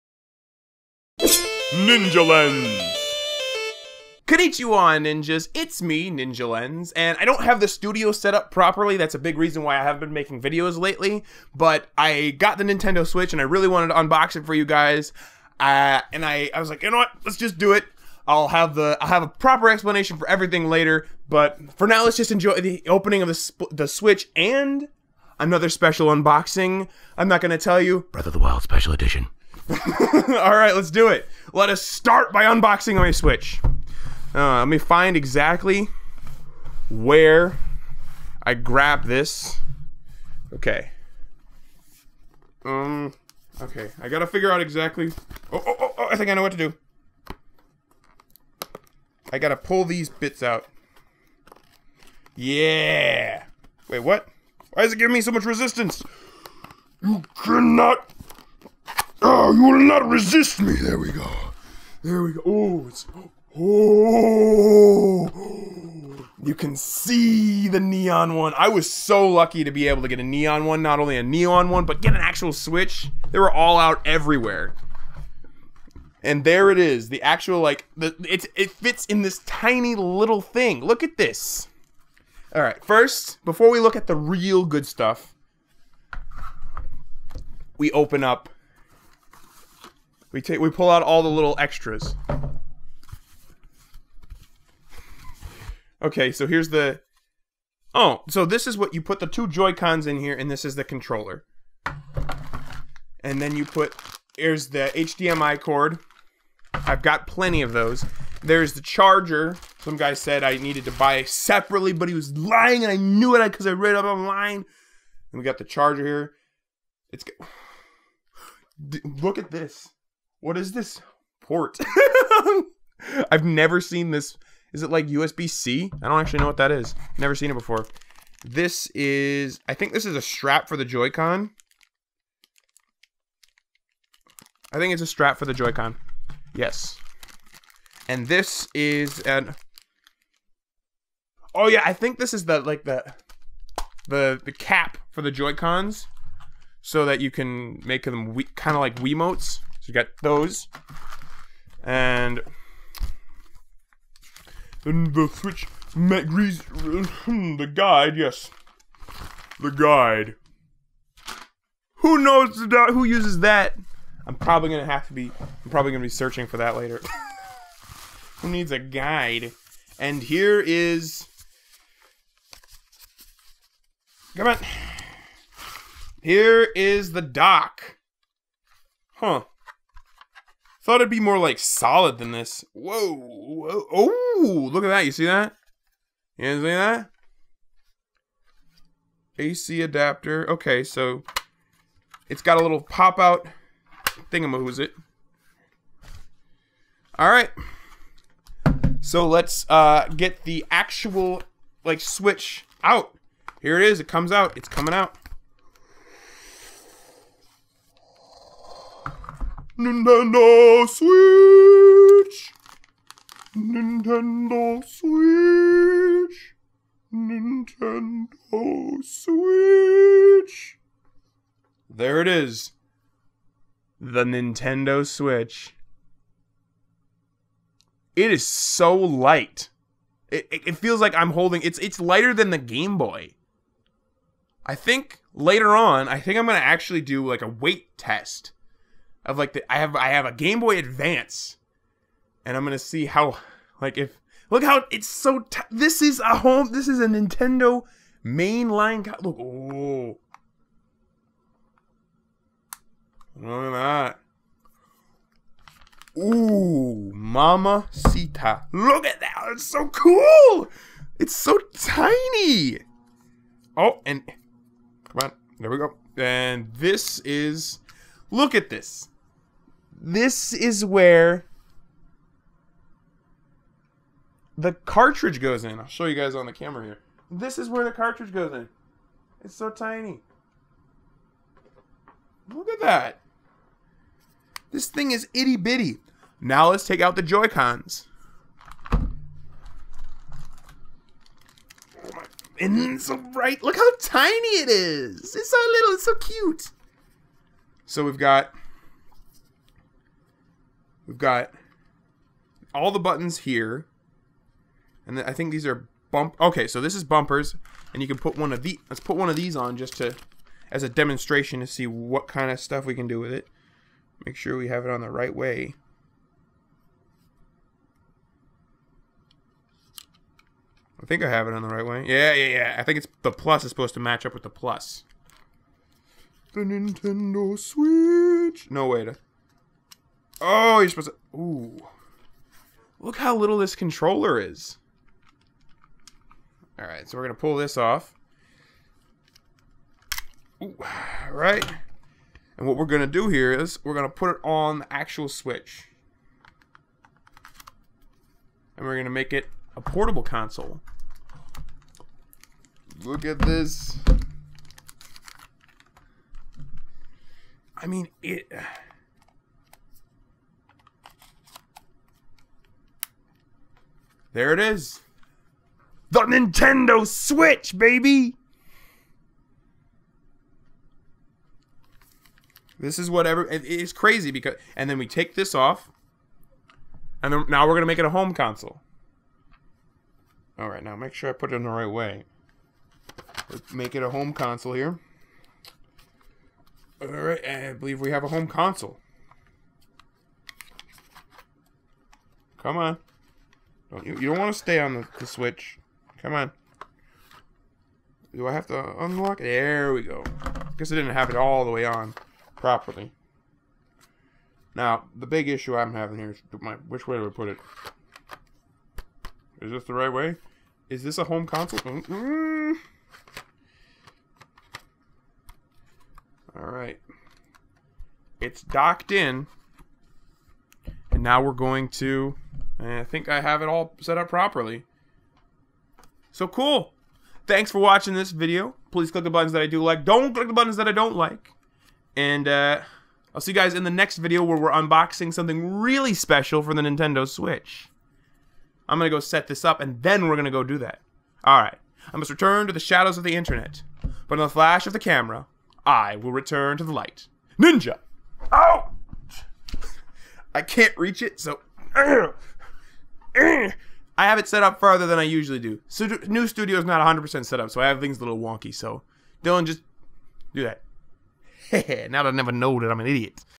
NINTENDO SWITCH UNBOXING!!!(neon)
managed to get a nintendo switch before they ran out SO LETS UNBOX IT!